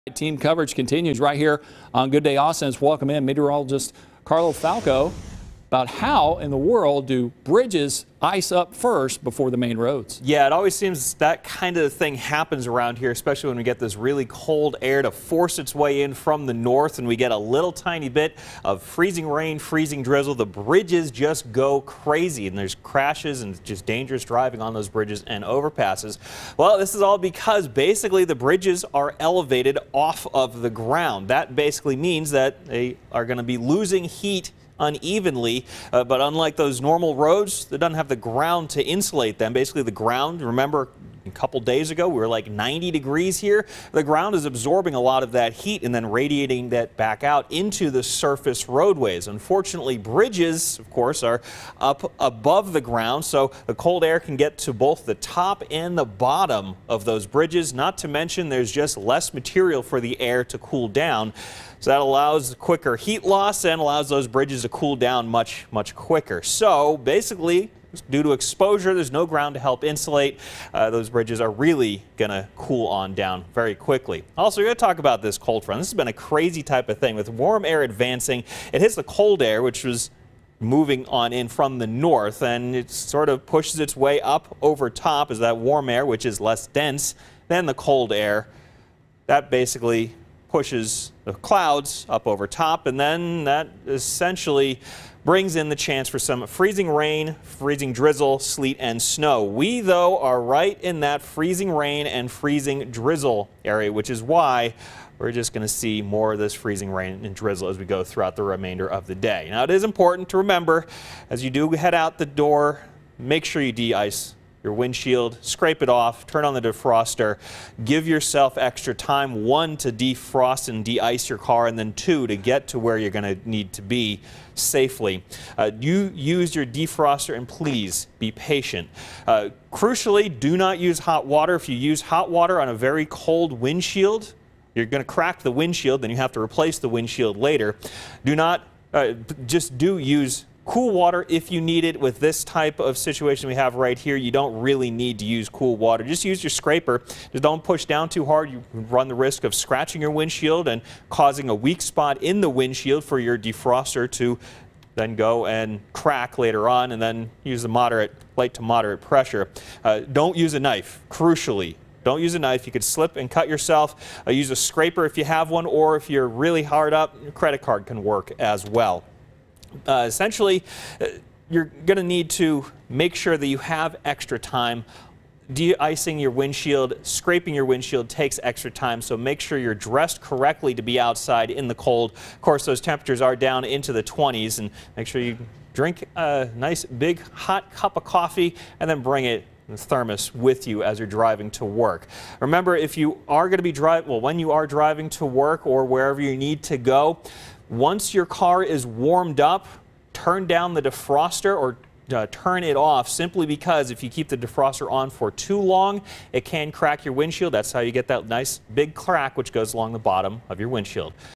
Winter weather: Why bridges freeze before surface roads | FOX 7 Austin
FOX 7 Austin meteorologist Carlo Falco talks about why bridges freeze before surface roads and has some tips on deicing your car as well.